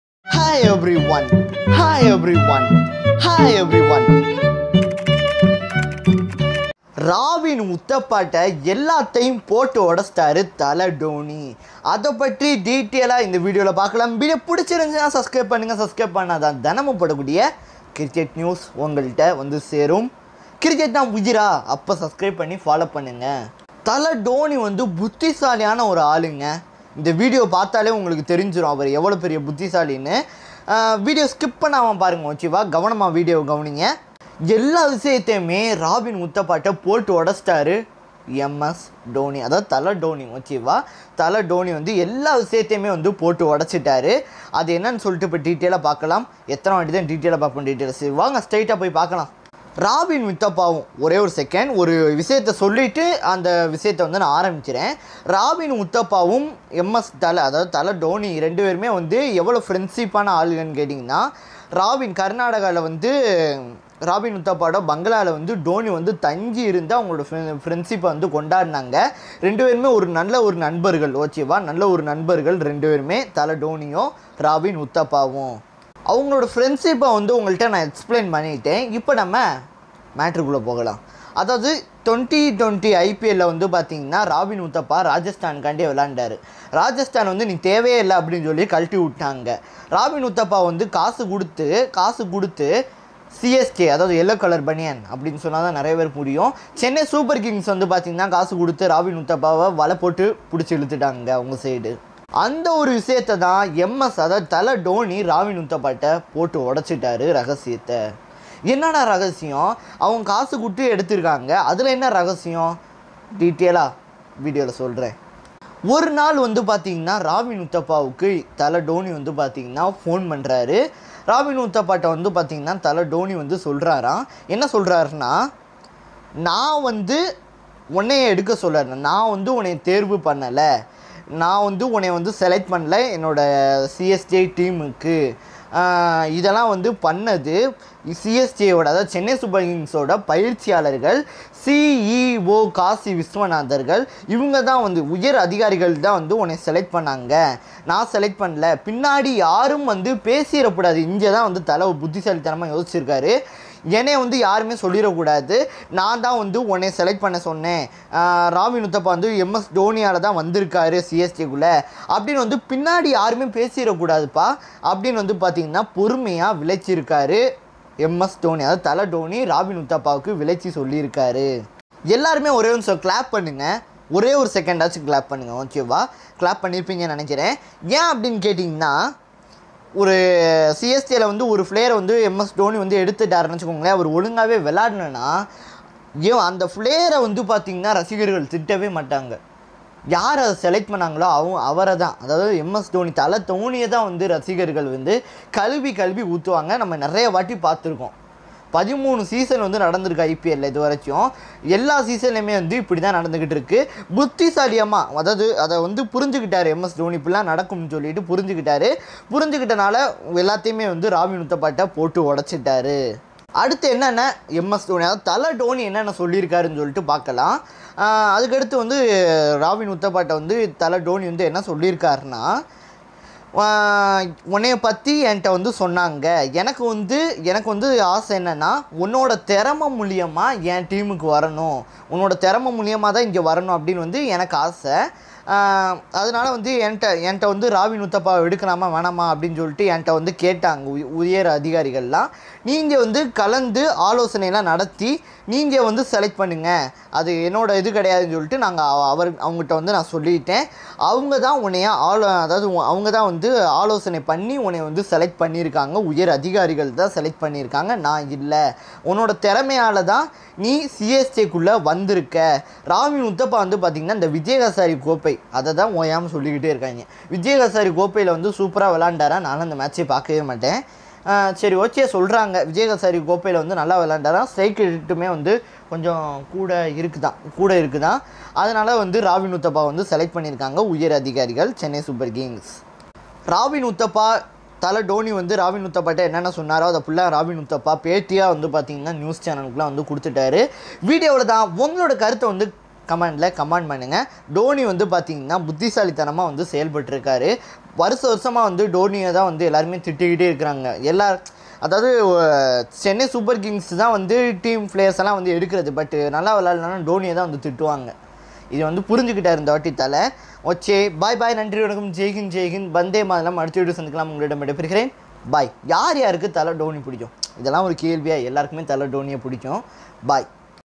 நான் உன்னை தேர்வு செய்யவில்லை csk MS Dhoni#Robin Uthappa#video
நான் உன்னை தேர்வு செய்யவில்லை
csk MS Dhoni#Robin Uthappa#video

IPL 2021 CSK Dhoni | நான் உன்னை தேர்வு செய்யவில்லை: ராபின் உத்தப்பாவிடம் கூறிய சிஎஸ்கே கேப்டன் தோனி

IPL 2021 CSK Dhoni | I did not choose you: CSK Captain Dhoni told Robin Uthappa